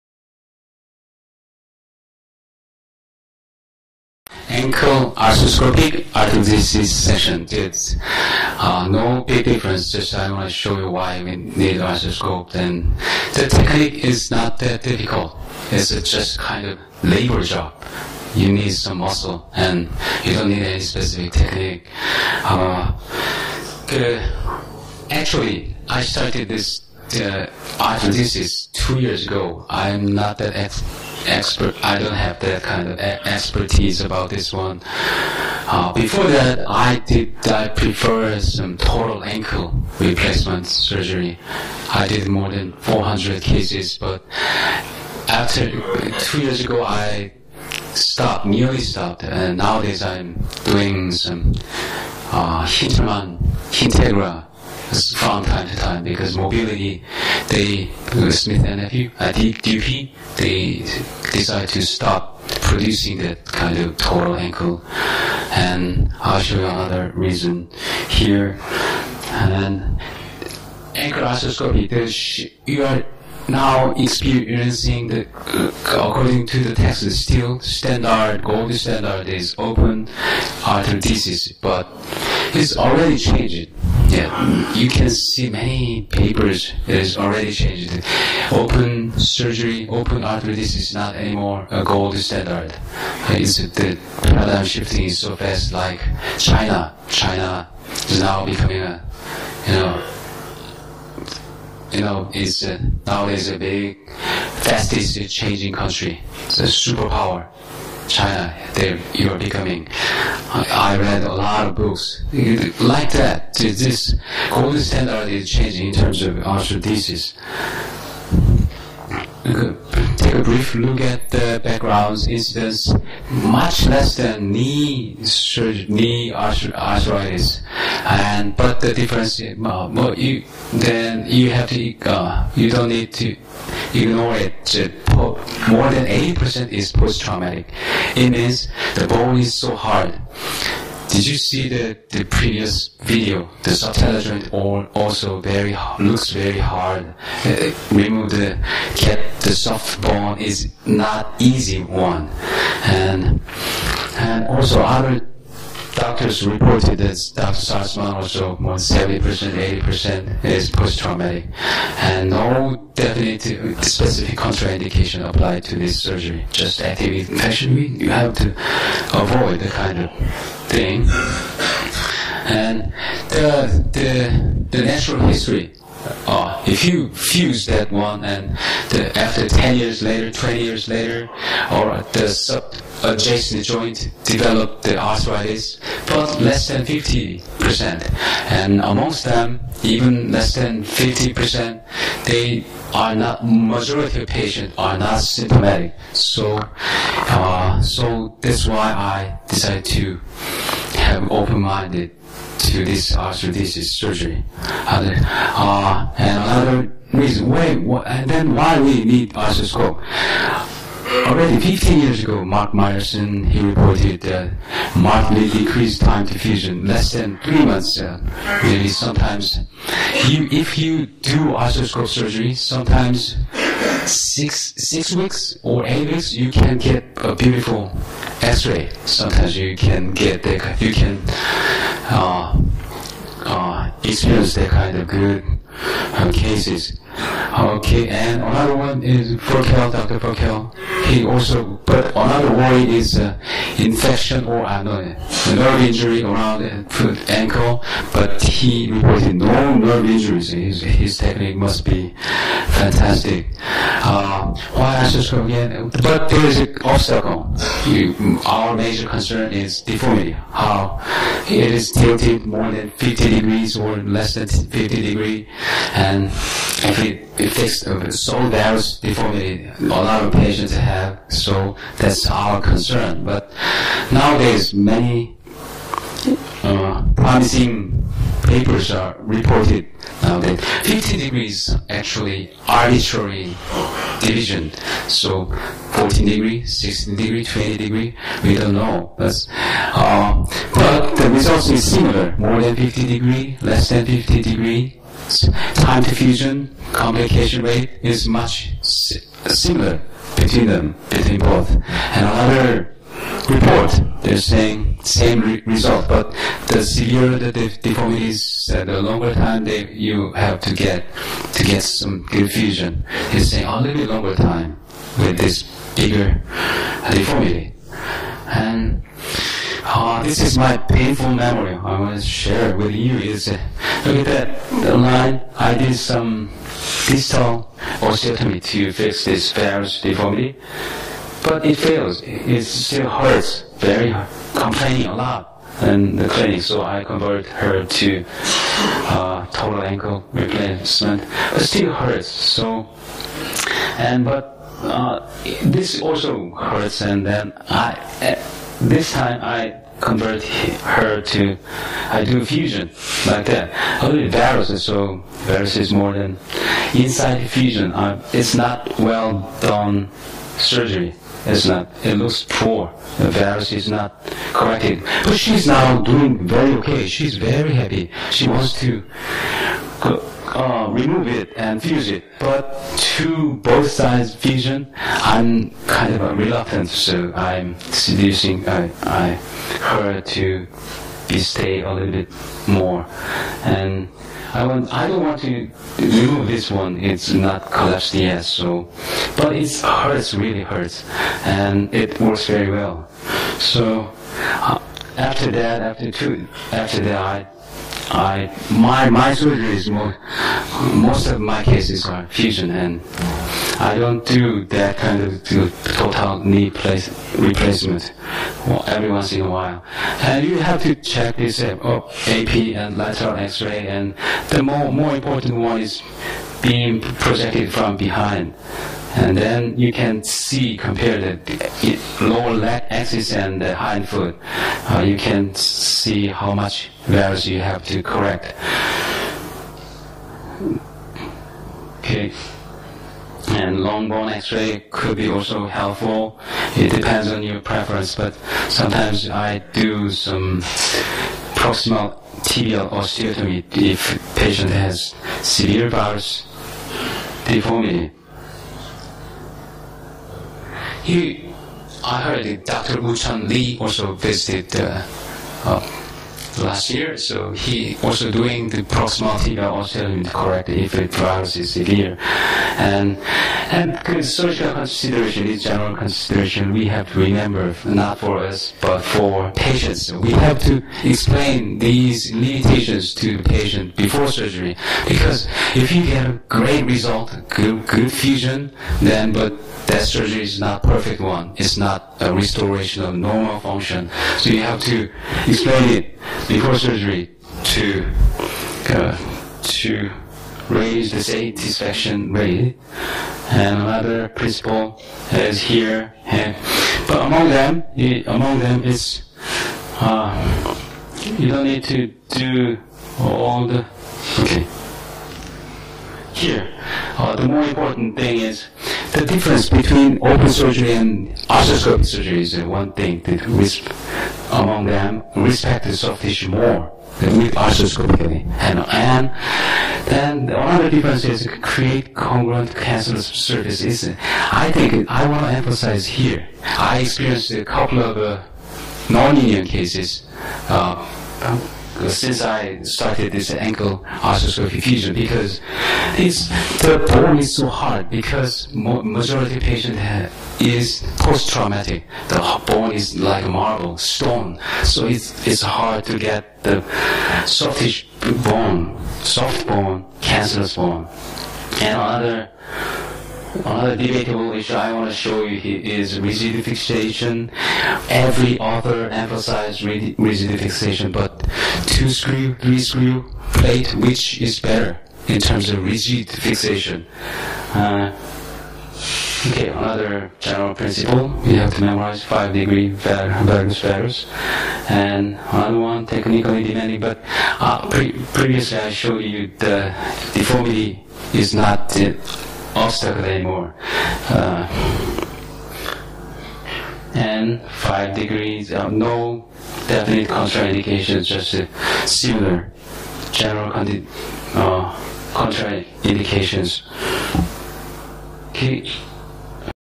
광명새움병원 김응수 원장 상하이 발목관절내시경 발목관절유합술_01
국내 유일, 상하이 족부학회에 초청된 김응수 원장님의
발목관절내시경을 이용한 발목관절 유합술
arthroscopic ankle arthrodesis

첫번째 영상